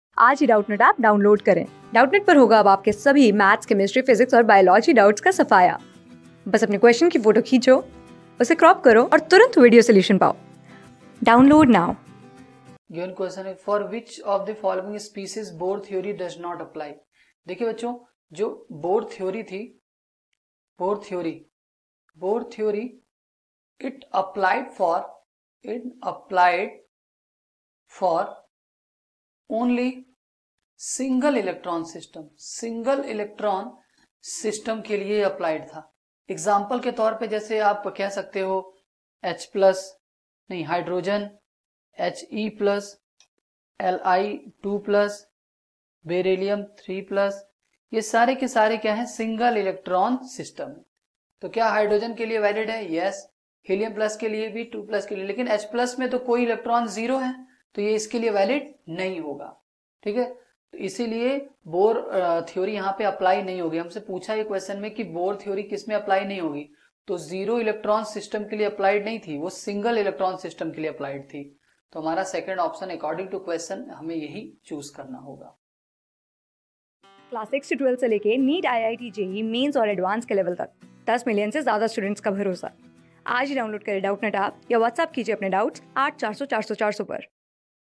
For which of the following species, Bohr theory does not apply? | CLASS 12 | ATOMIC STRUCTURE AN...
For which of the following species, Bohr theory does not apply?
Class: 12
Subject: CHEMISTRY
Chapter: ATOMIC STRUCTURE AND RADIOACTIVITY
Board:IIT JEE

👉 Have Any Query? Ask Us.